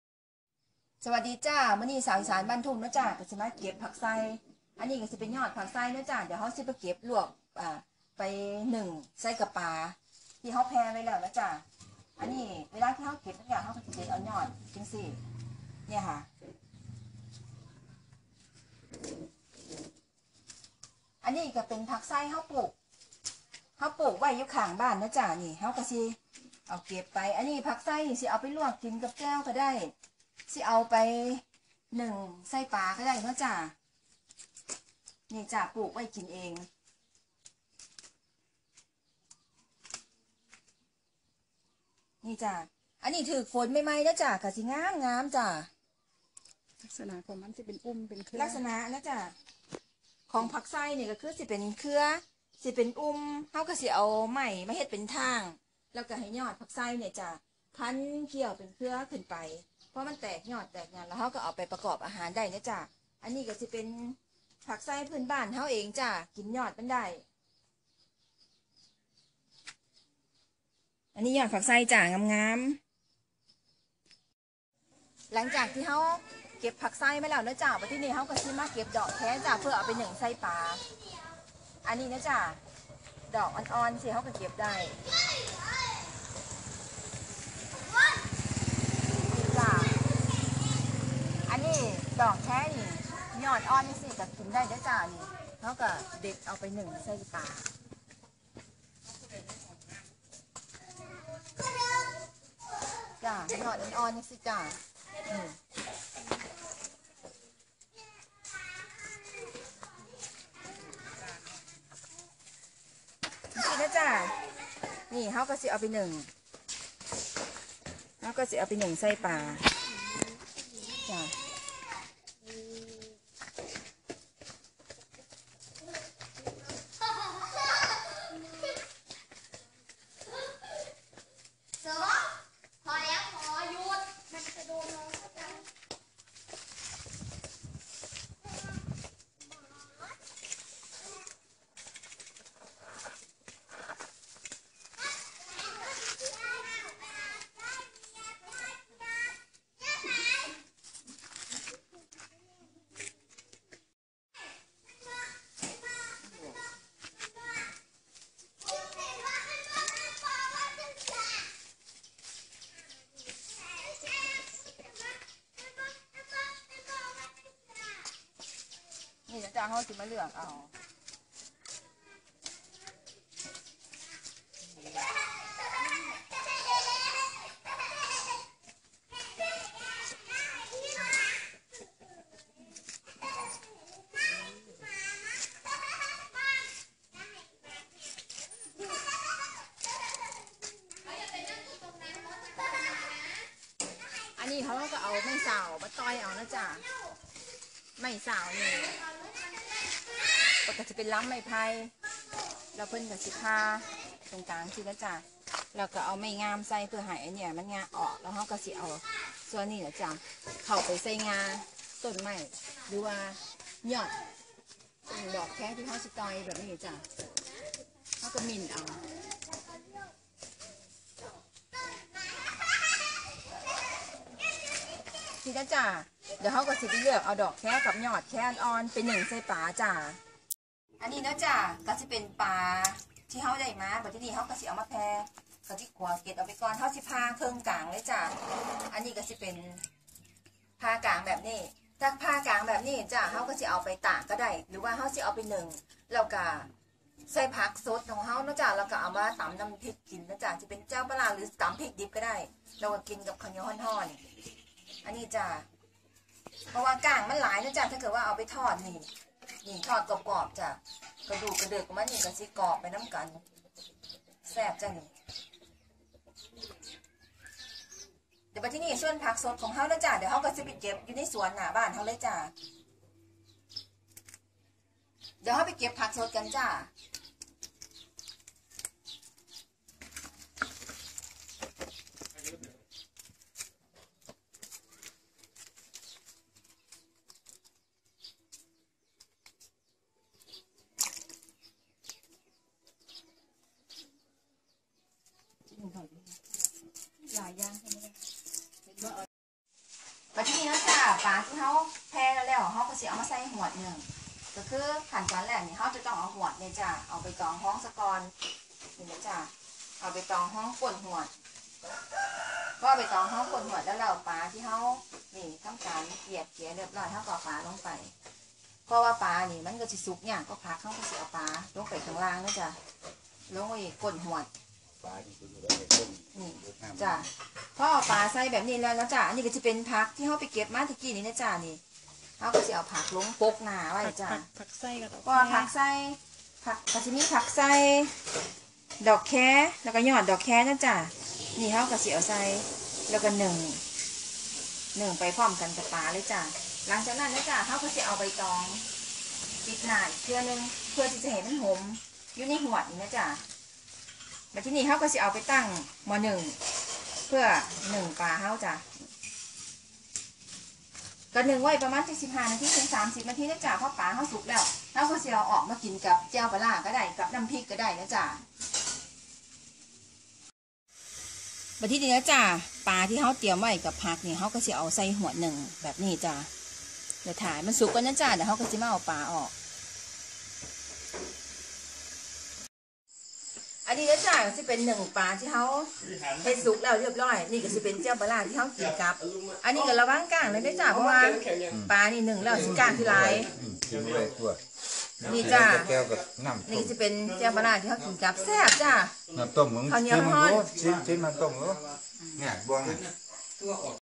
นึ่งปลาหมักเกลือ สูตรสาวอีสานบ้านทุ่ง
นึ่งปลาขาวหมักเกลือ หรือ เอียบเกลือ นึ่งใส่ผักไส่,ยอดดอกแค,ดอกแค เก็บเอาตามบ้านเฮานั่นหล่ะ สุกแล้วกะกินกับแจ่ว เข้ากันคักจ้า มาเด้อมากินนำกันจ้า